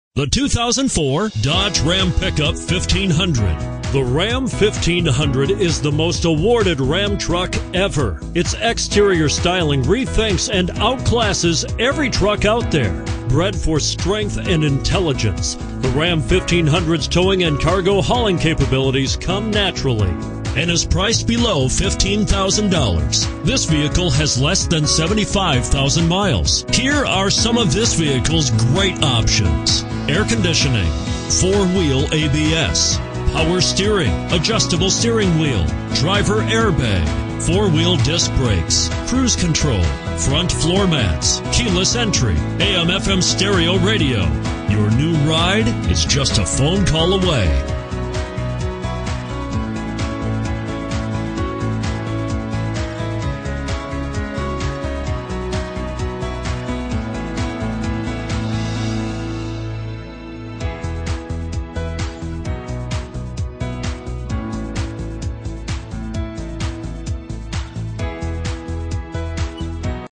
2004 Dodge Ram Concord CA 1836
2004 Dodge Ram  - Stock#: 1836

Looking for the right car? Today could be your lucky day. With 70554 miles, this , 2004 Dodge Ram equipped with Automatic transmission could be yours. Request more information and set up a test drive right away.